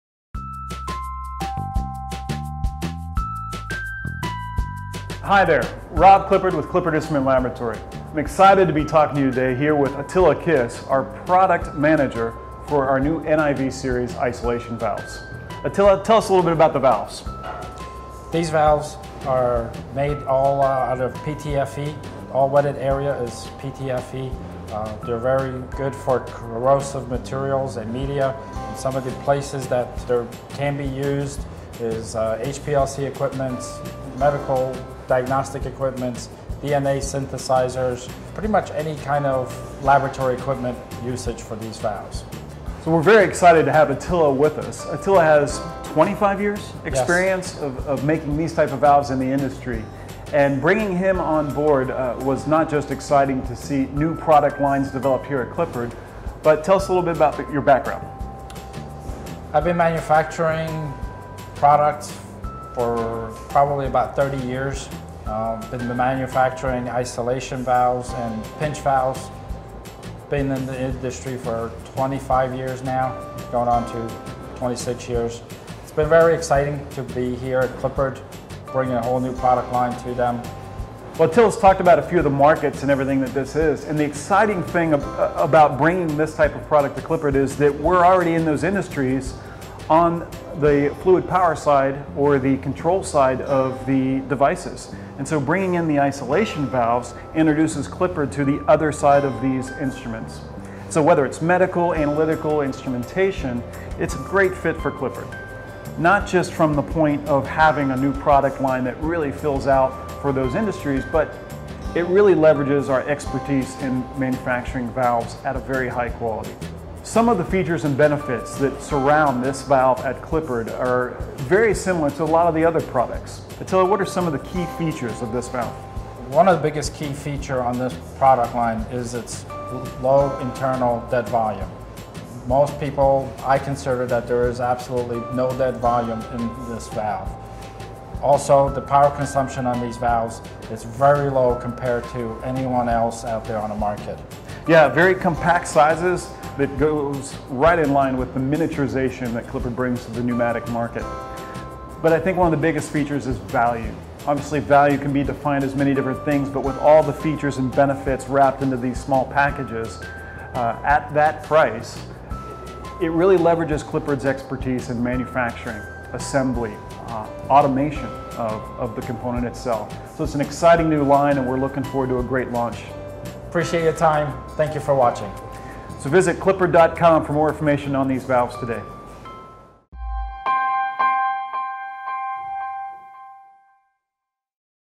Clippard’s expanding medical instrumentation: the new NIV isolation valve
Clippard Instrument Laboratory's Vice President Rob Clippard and Attila Kiss, NIV/NPV Product Manager, introduce the company's new NIV series of media isolation valves and how this new series allows Clippard to bring its valve expertise from the fluid power and control side of medical and laboratory applications to the other side of these devices for dispensing of media.